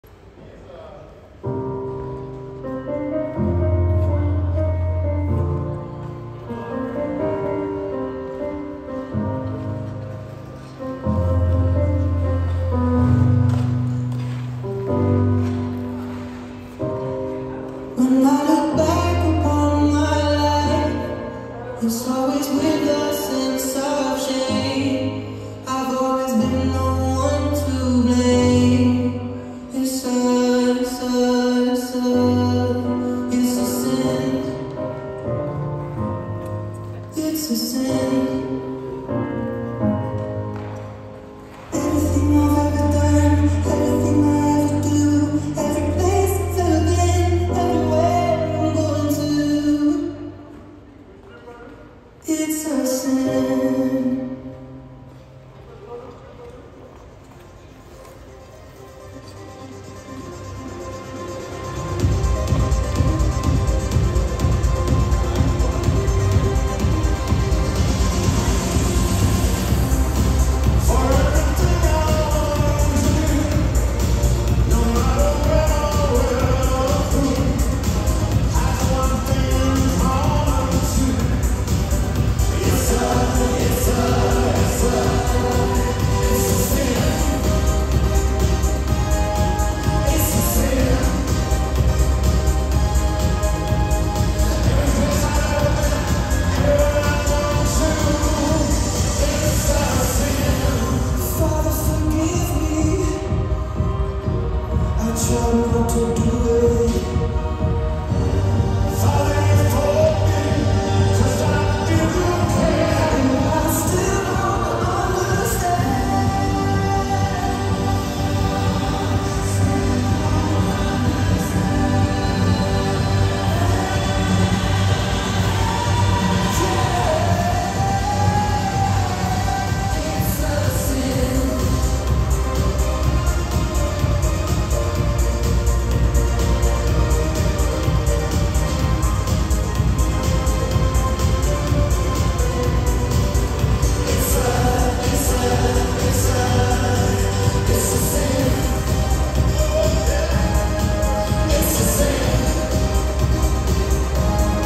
Music on Ice 2022 Rehearsal
You may not use our content in any derivative work (including, but not limited to, short clips, GIFs, and edited videos).
ミュージック・オン・アイスのリハーサルより。2022年4月15日、ベリンツォーナ・スポーツセンターにて、主催のトーベル氏に許可を頂いています。
Music on Ice 2022 practice on 15 Aprile 2022 at Bellinzona Sport Center.
I asked Mr. Tobel for permission to film the video because the rehearsal was not open to the public!